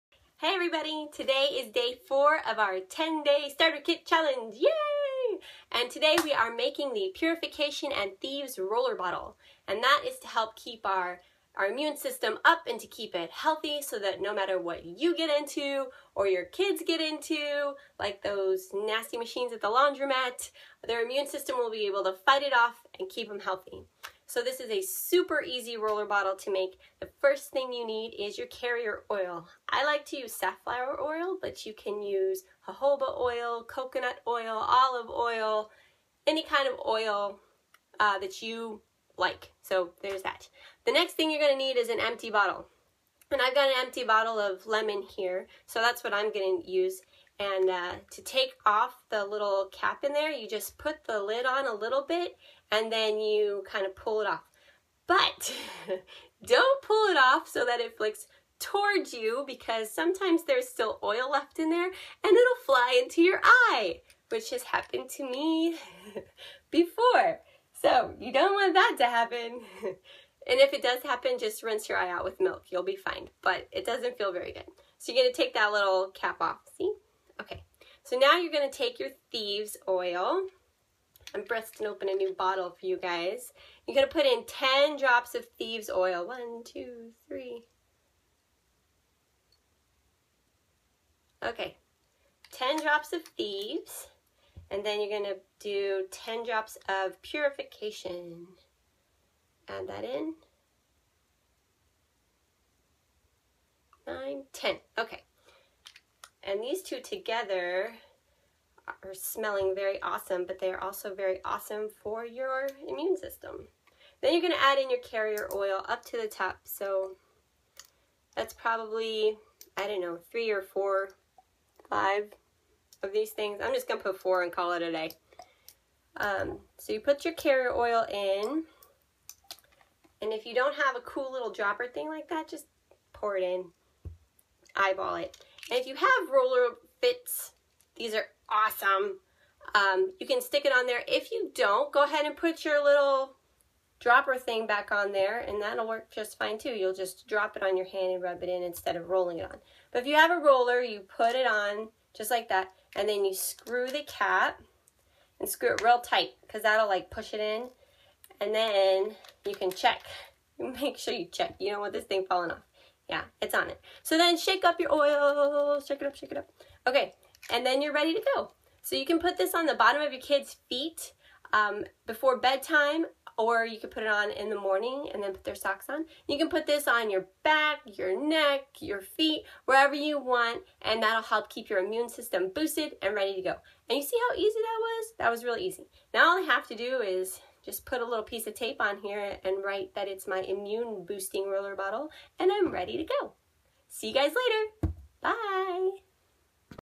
Immune Boosting Roller Bottle
You never know what you or your kiddos are going to get into on a daily basis. This blend of Thieves and Purification boosts your immune system to keep it in top notch germ fighting form. So no matter what you come across, you'll be ready to fight it off!

If you want more information about the power of essential oils, check out my FB

If you're ready to get some butt kicking oils of your own, you can sign up